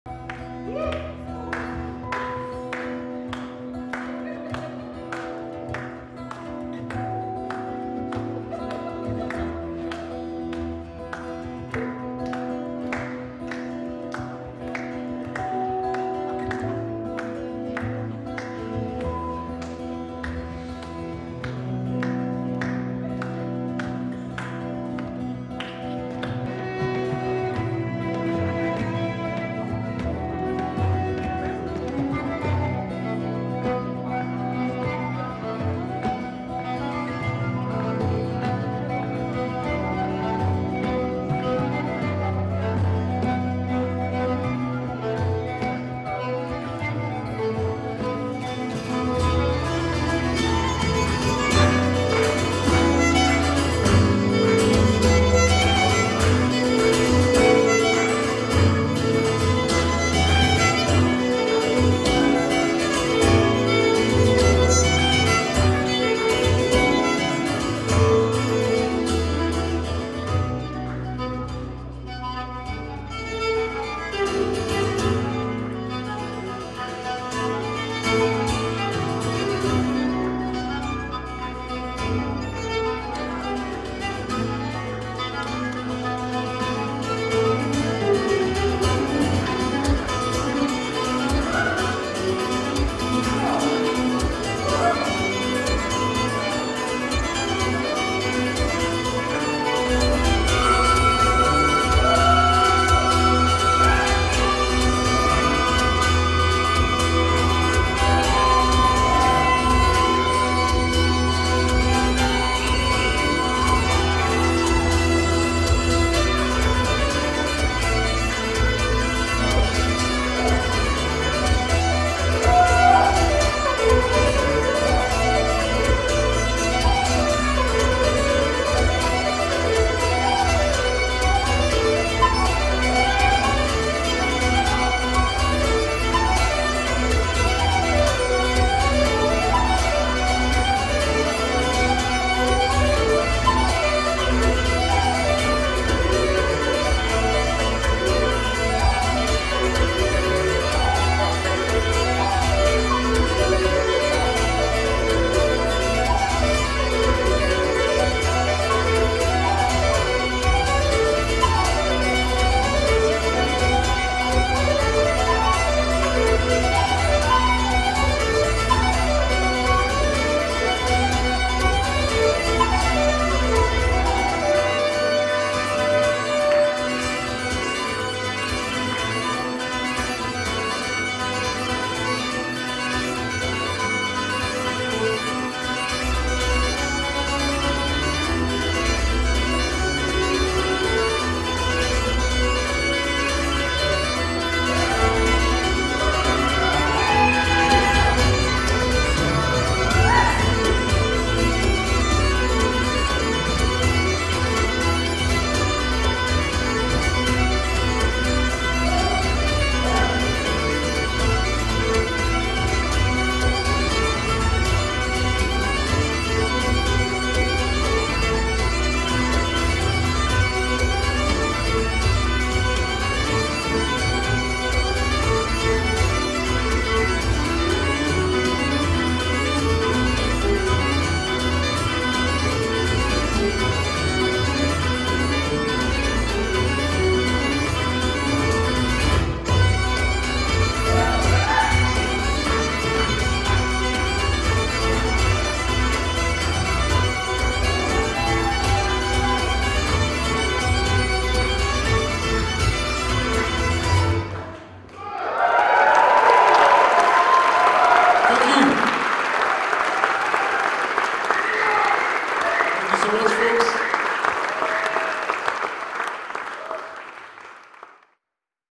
RURA
This is RURA who played a great sellout set in St Margaret's on Friday night March 31 2023